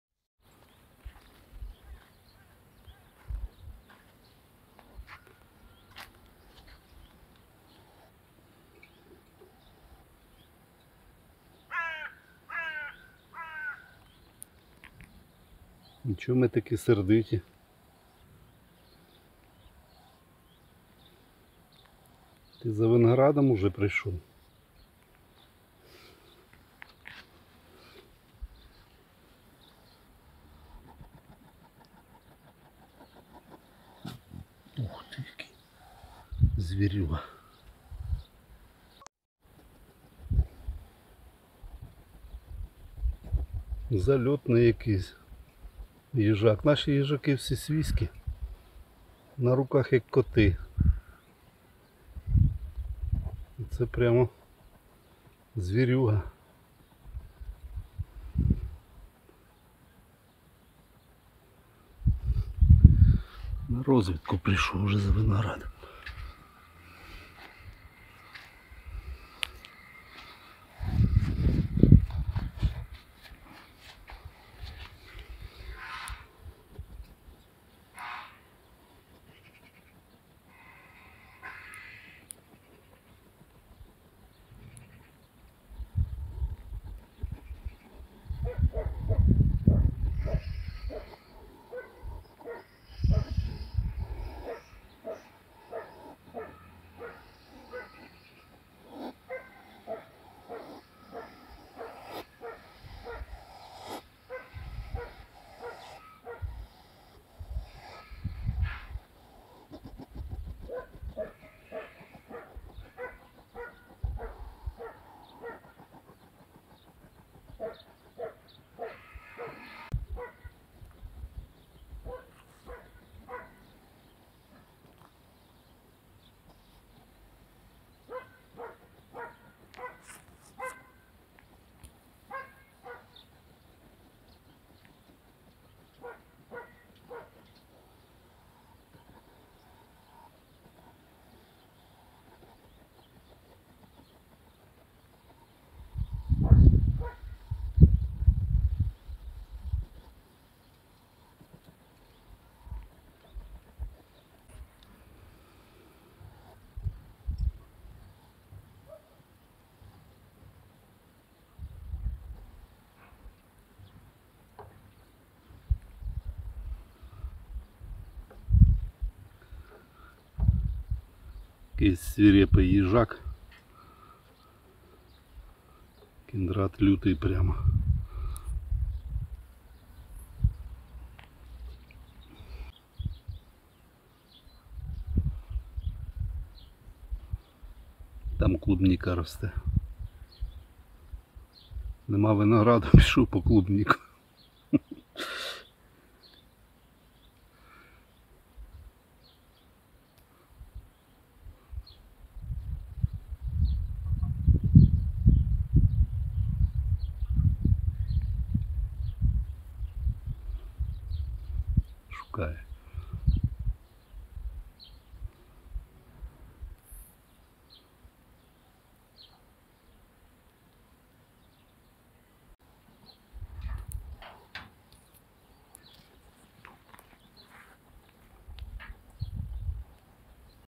Перший невдоволений клієнт на виногр@днику.
розлючений їжак на виногр@днику #